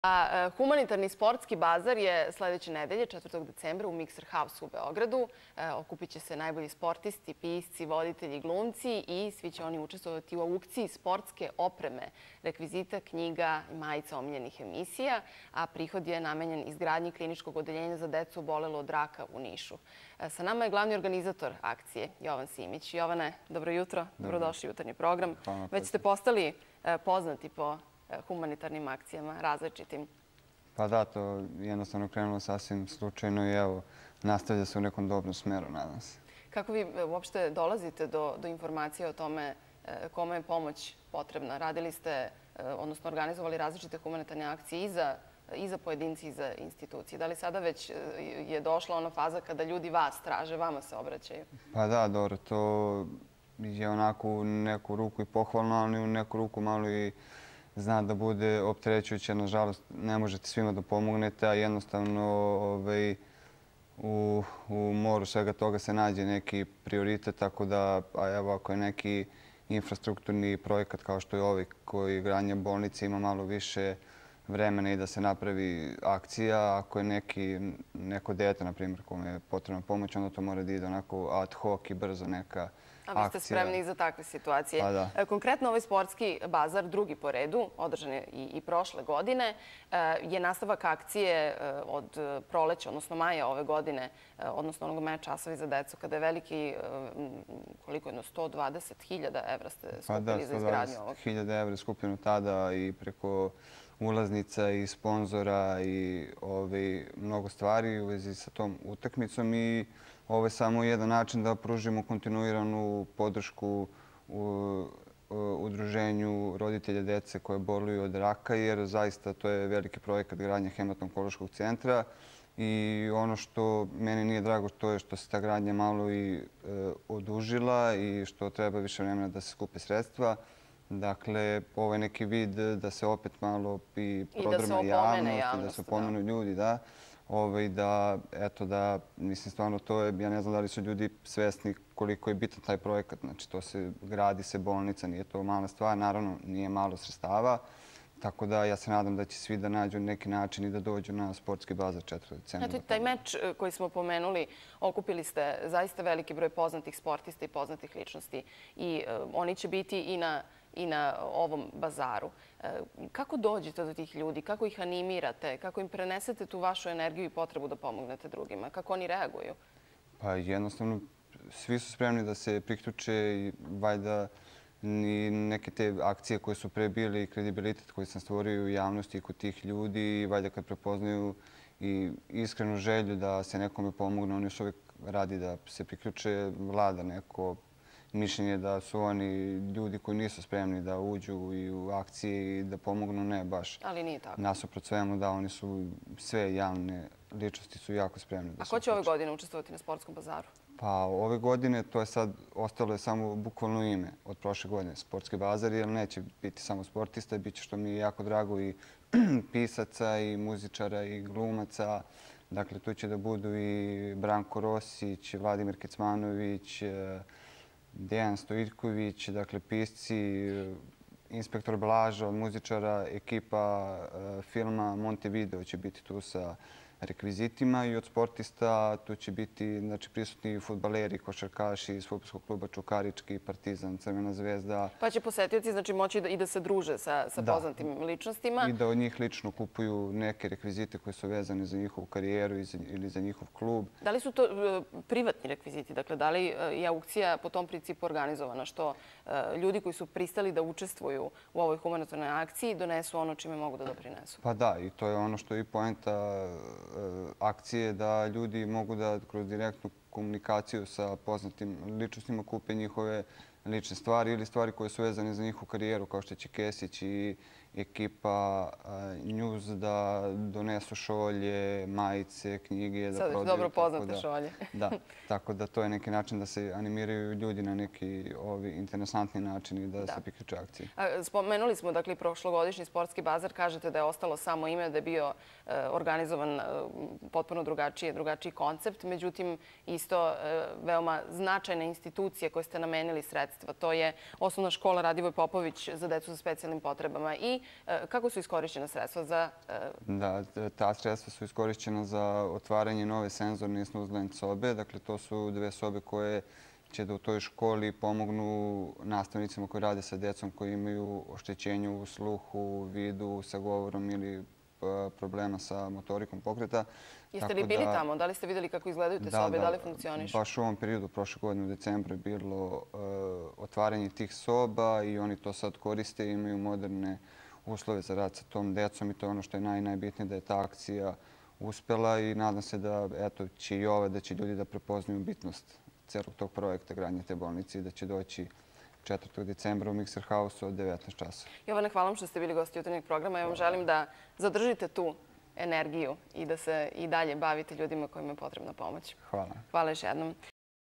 Sportski bazar
Humanitarni "Sportski bazar'' je sledeće nedelje, 4. decembrau Mikser Hausu u Beogradu. Najbolji sportisti, pisci, voditelji i glumci  učestvovaće u aukciji sportske opreme, rekvizita, knjiga i majici  omiljenih emisija. Prihod  je namenjen izgradnji kliničkog odeljenja za decu obolelu od raka u Nišu. Sa nama je  glavni organizatori akcije  Jovan Simić.